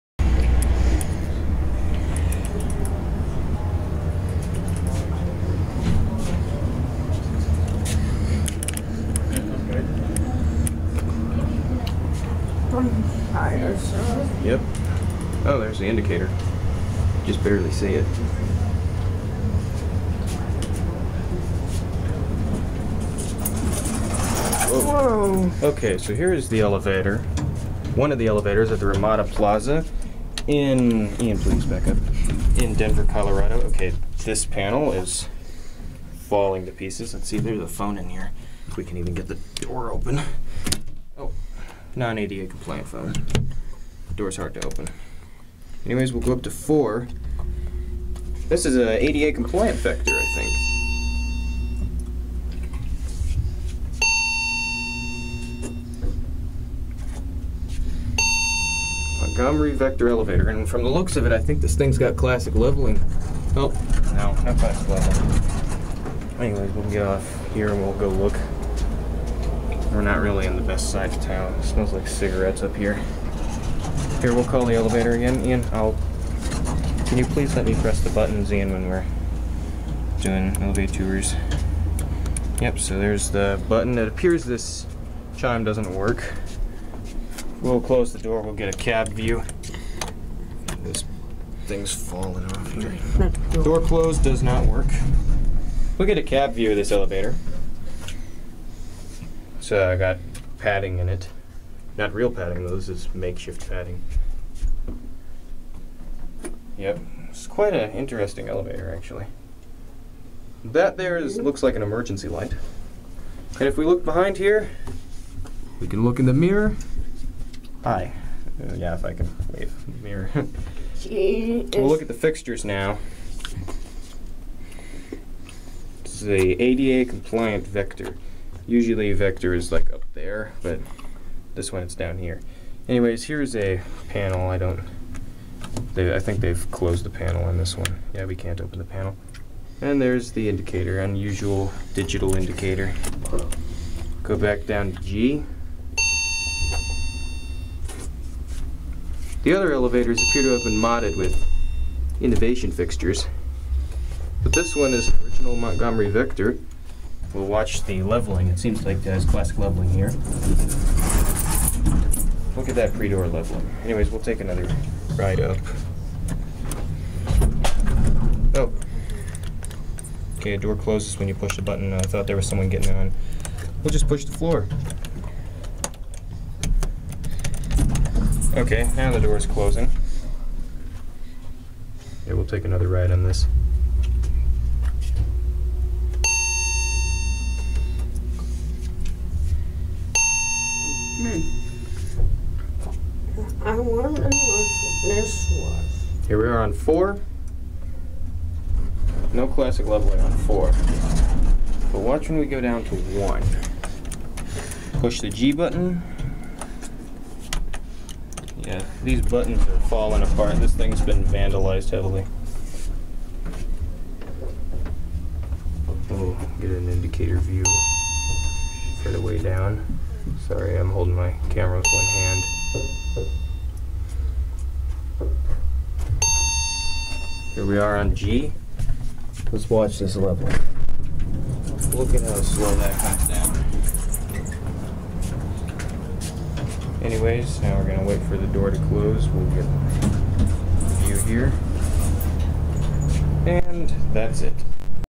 Montgomery Hydraulic Elevator at the Ramada Plaza, Denver, CO
This is a rare ADA-Compliant Montgomery Vector. This is only the third Vector I've ever found, the first being at my school and the second at another school, both of those are non-ADA-Compliant. I assume this elevator is getting modernized soon though, the other two elevators here recently got a cheap Innovation mod. The specifications can be found below.

Brand: Montgomery
Motor Type: Hydraulic
Modernized: No (Might be soon)
Hall Stations: Montgomery Vector
-Illumination Color: Green
-Button Type: Standard
-Panel Finishing: Stainless Steel
Hall Lanterns: Montgomery Vector
-Illumination Color:
  -Up: Orange
  -Down: Orange
-Lantern Style: Vector (Blue VF indicator on Level 1)
Cab Stations: Montgomery Vector
-Illumination Color: Green
-Button Type: Standard
-ADA Compliant: Yes (This is rare!)
Indicator: Montgomery Vacuum Fluorescent Dot Matrix
-Illumination Color: Blue/Teal
Cab Lanterns: Montgomery Vector
-Illumination Color:
  -Up: Orange
  -Down: Red
-Lantern Style: Horizontal Door Lantern
Phone: Generic
-ADA Compliant: No
-Location of Phone: In Cabinet
Door Type: Single Slide
-Safety Sensor Type: Infrared
Capacity: 3000 Lbs
Speed: 150 FPM
Distance Traveled: 40 Feet
Floors Served: *G, 2, 3, 4
Year Installed: 1990 or later
Other Observations or Details: The fixtures are falling off the wall.